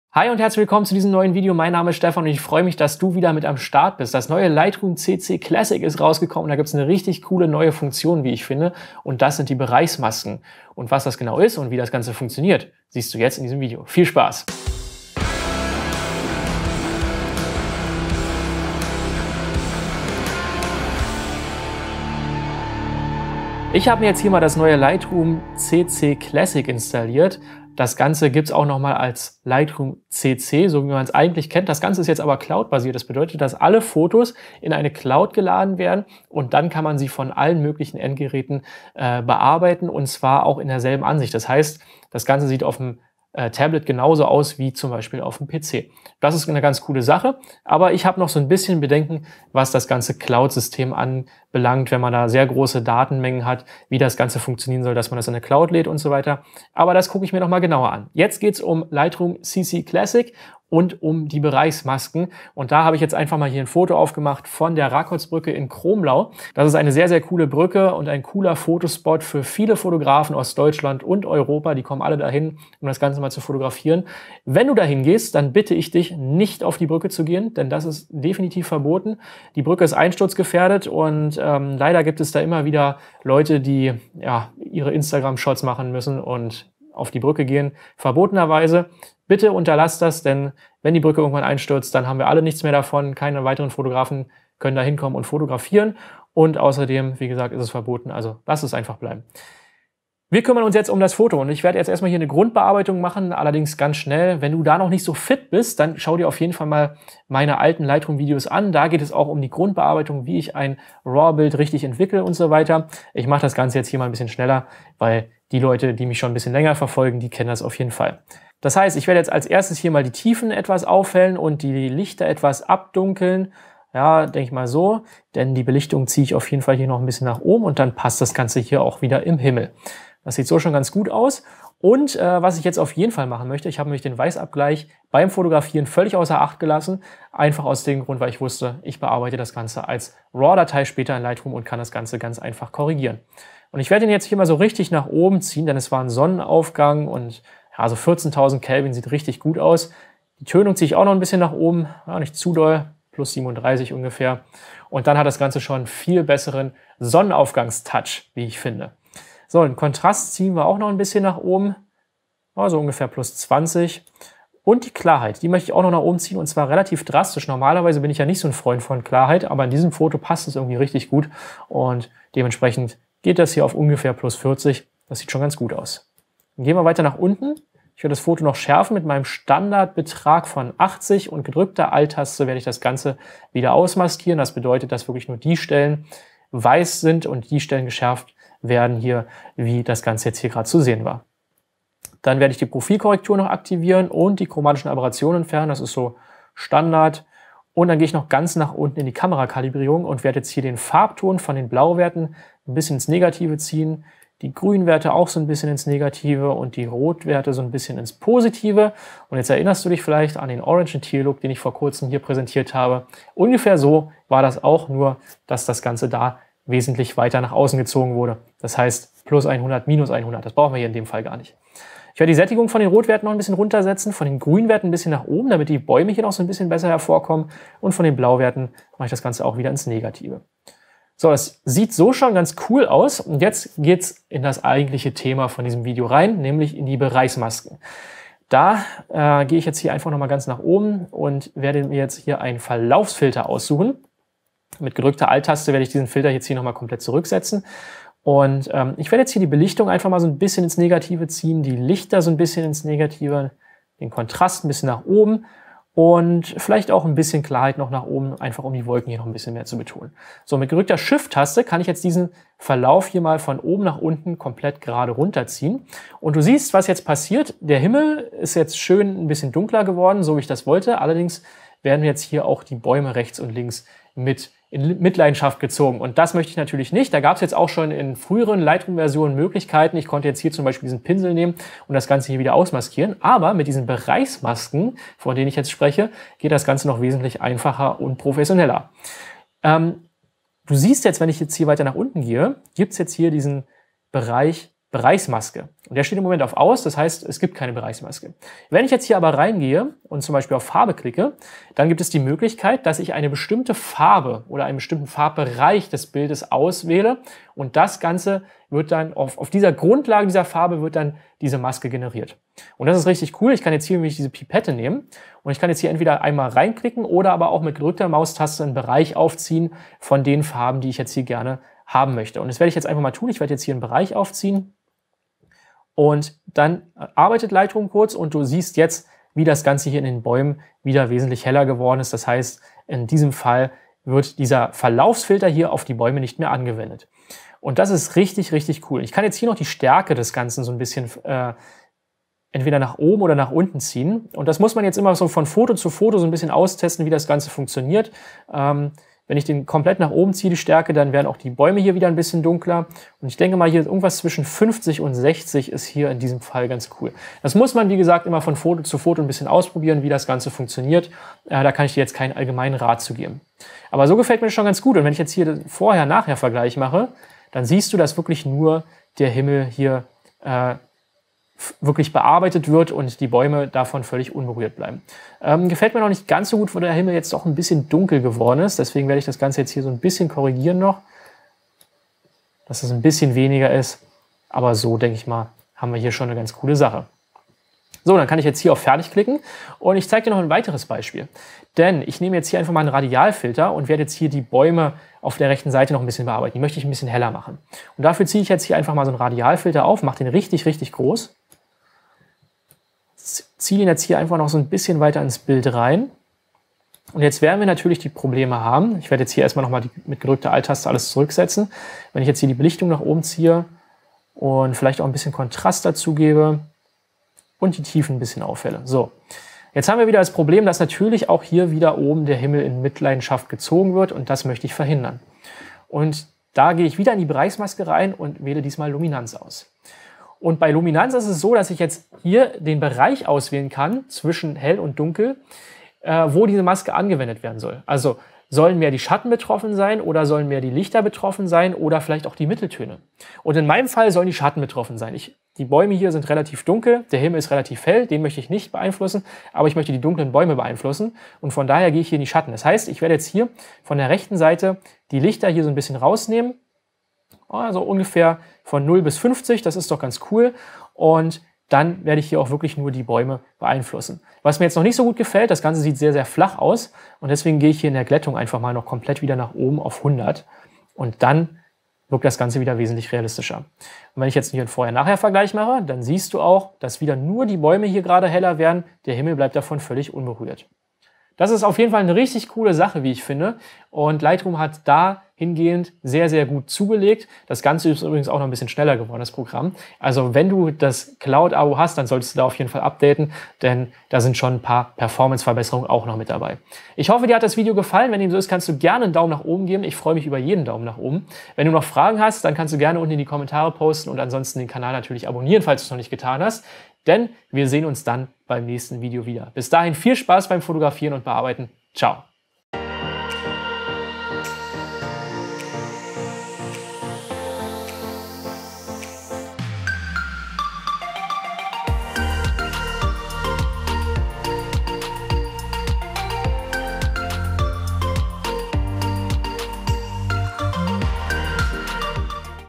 BEREICHSMASKEN IN LIGHTROOM (Luminanz & Farbe) | Tutorial Deutsch
Das neue Lightroom bietet dir Bereichsmasken auf Grundlage von Farbe und Luminanz! Wie es genau funktioniert, zeige ich dir hier!